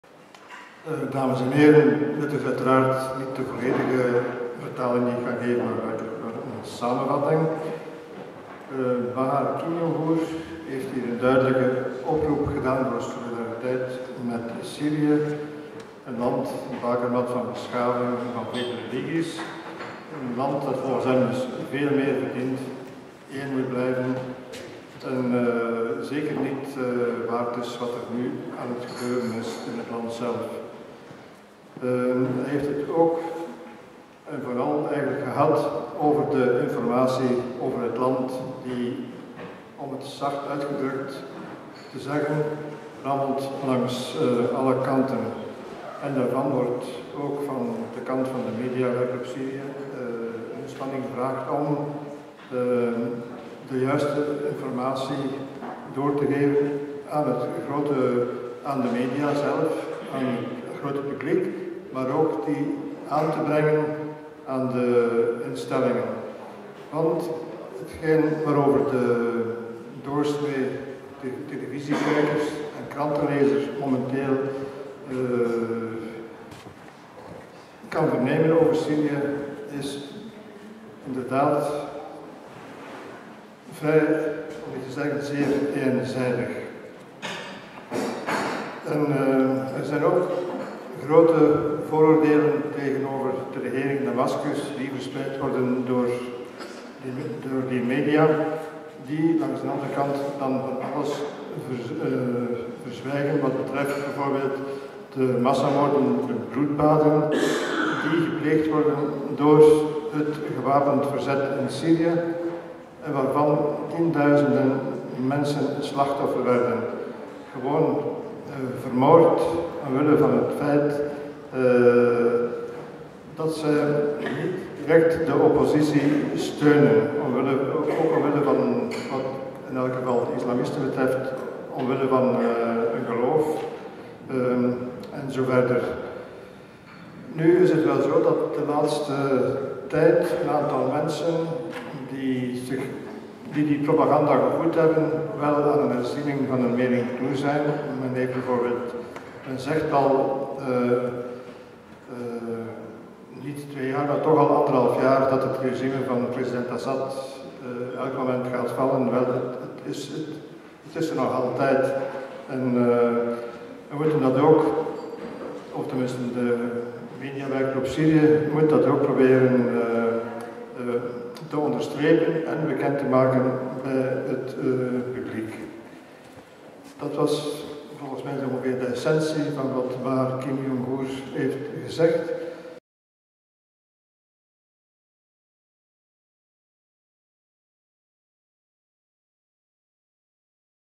Kimyongür Vertaling: Onderzoek oorlog in Syrië NL CONF13
Bahar Kimyongür vertaling: Klachten en Onderzoeken rond de oorlog in Syrië. Vertaald vanuit het Frans door Paul Vanden Bavière.

This video was recorded at the conference from the Media Workgroup Syria (MWSyria.com) in Brussels, March 2013 with consent of the speakers.